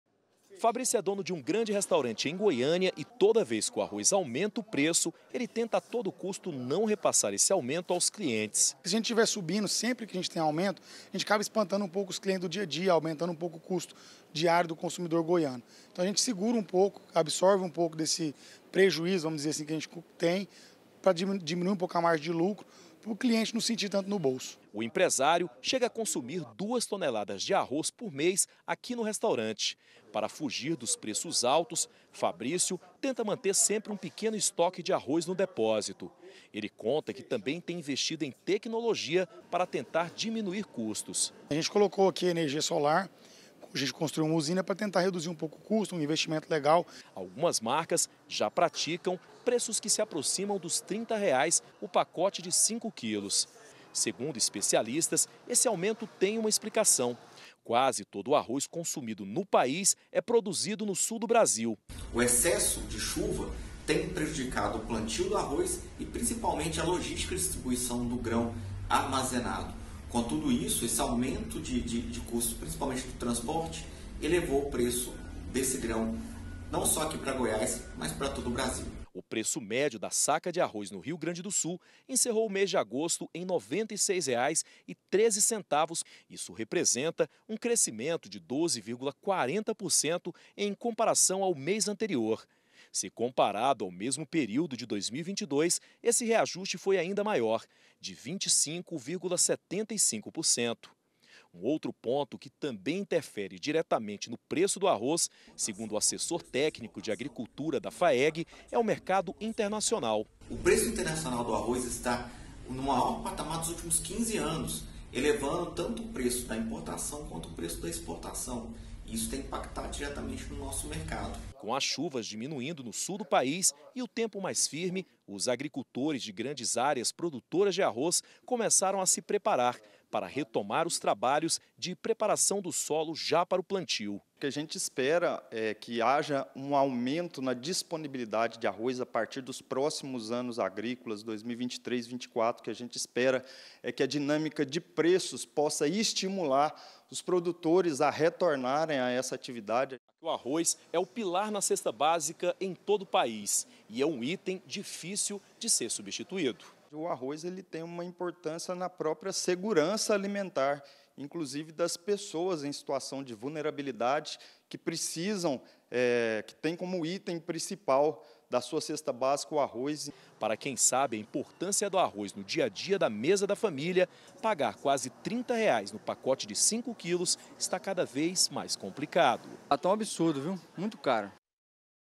EFEITO CHUVA: PREÇO DO PACOTE DE ARROZ DISPARA
O PREÇO DO ARROZ DISPAROU. O PACOTE DE CINCO QUILOS JÁ TÁ BEIRANDO OS TRINTA REAIS. O PROBLEMA SERIA A CHUVA, QUE QUEBROU A SAFRA DO RIO GRANDE DO SUL, MAIOR PRODUTOR DE ARROZ DO BRASIL.